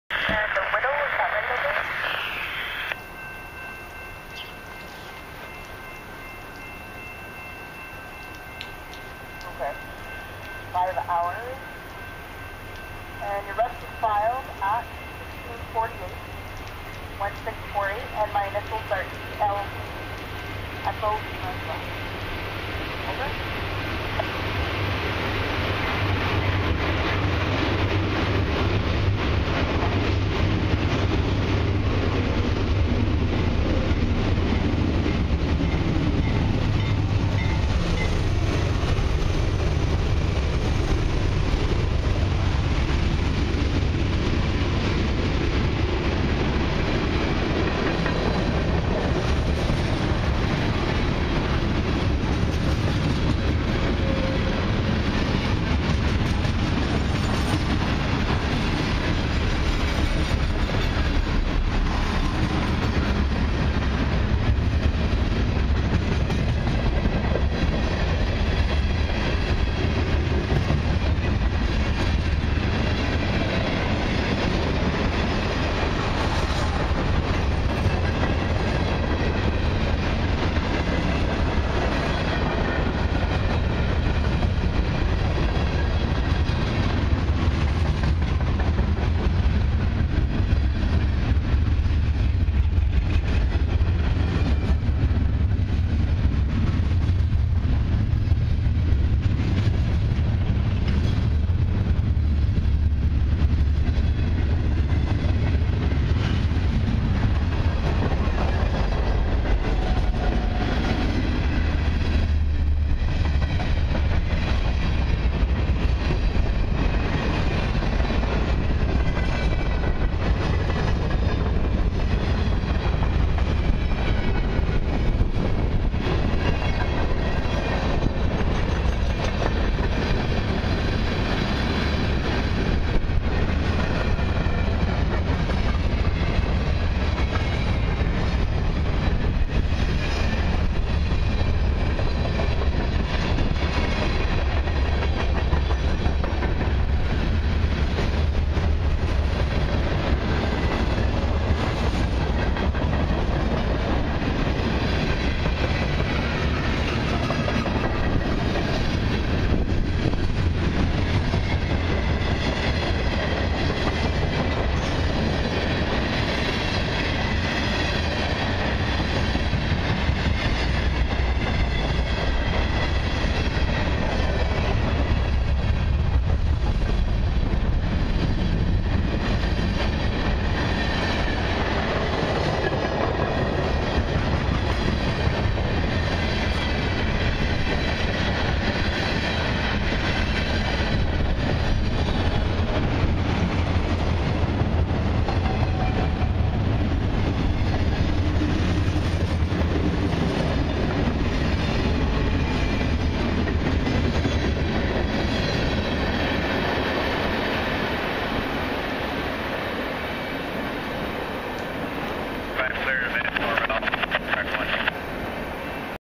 CN 435 w/ "Nude" CN 2132 & GECX's 7897 & 7893 lead this long 542 axle, WB Mixed/Autorack train by
.Brampton GO/VIA Station at the MP of 15.4 on the CN Halton Sub.

Here's my 7th catch from back on February 3rd. Thanks for the wave !

With CN 2132 being UP 9097 & CNW 8550. Also, with GECX 7897 being ex CSX 7897. And GECX 7893 being ex CSX 7893. Either ex CSX units are showing as ex Conrail units on the website that I check on.

video taken: Sunday February 3rd 2019 at 4:51pm
thanks ! =]